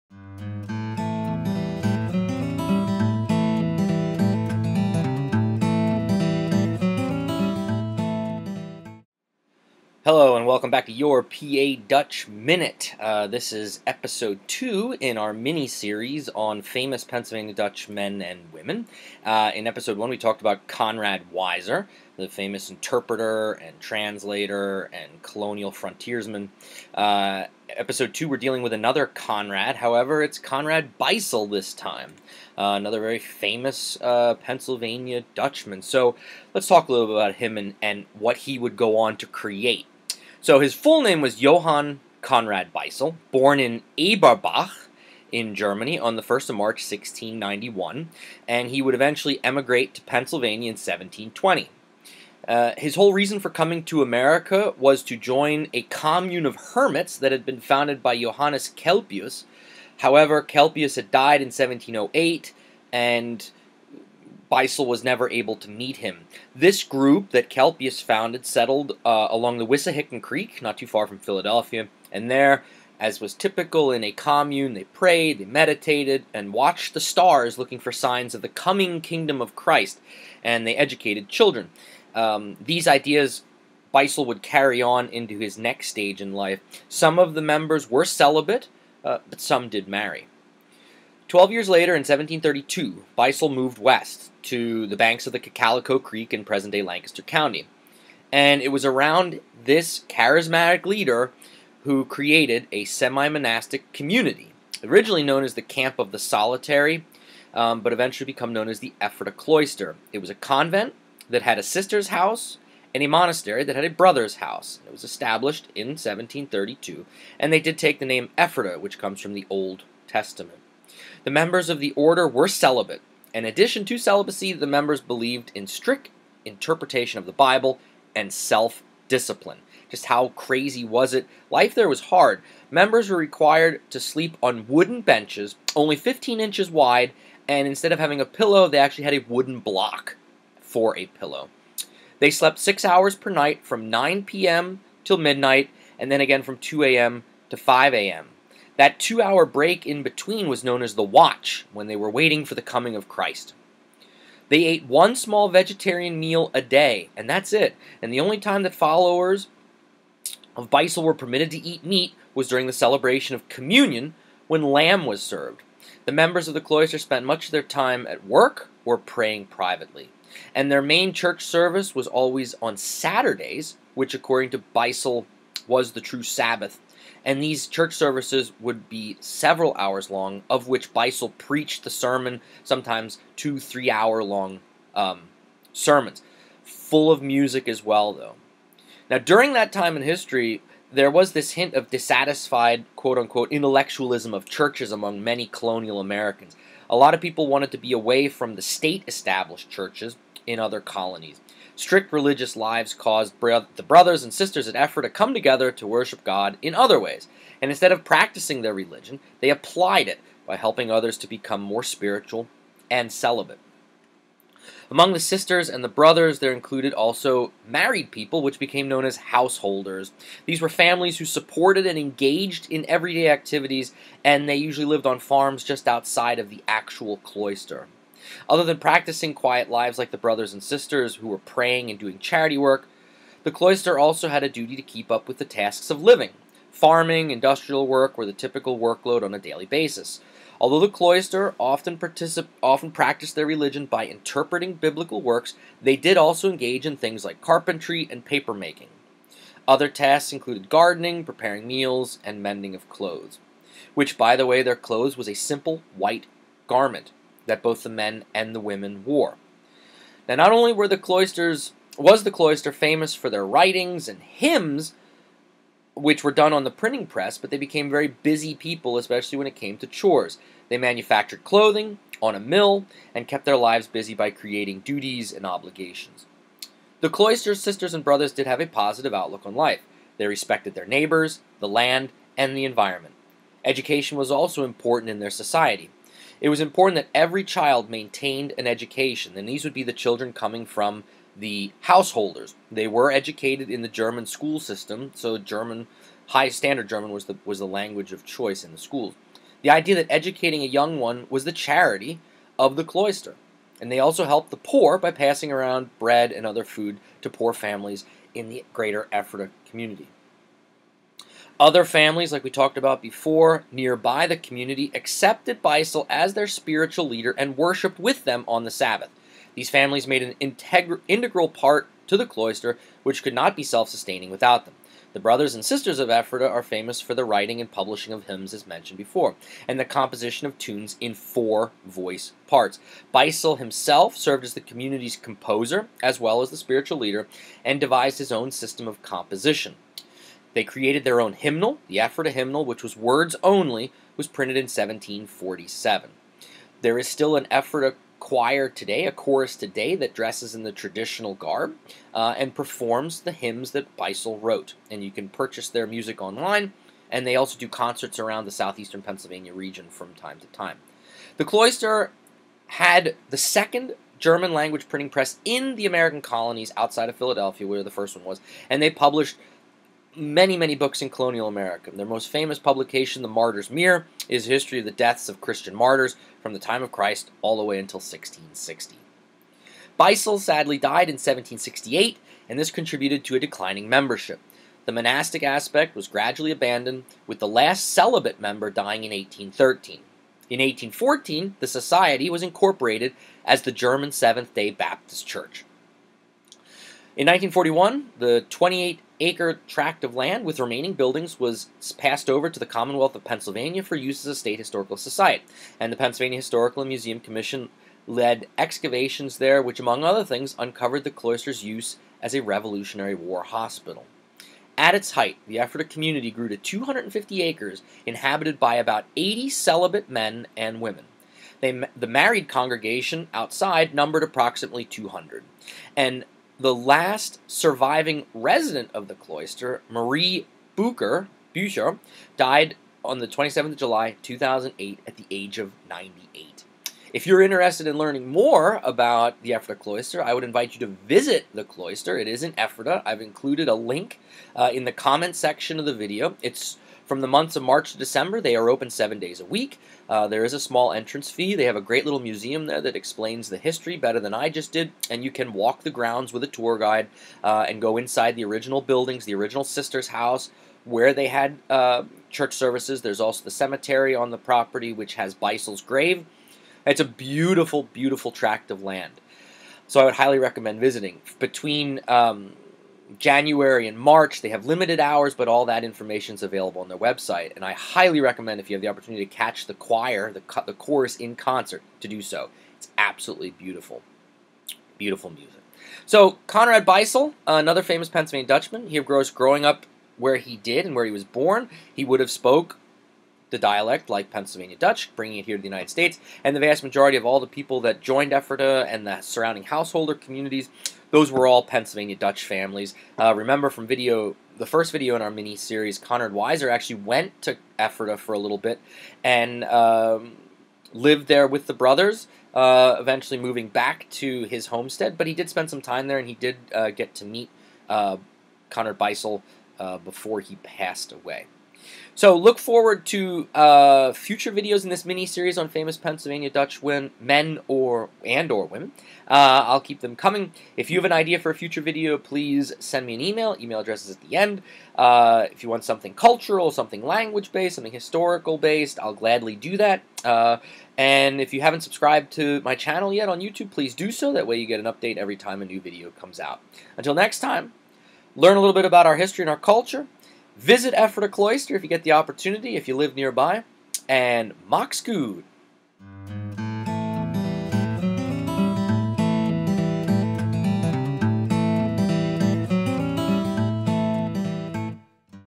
Your PA Dutch Minute: Conrad Beissel and the Ephrata Cloister
This episode explores the history of Johann Conrad Beissel and the Ephrata Cloister.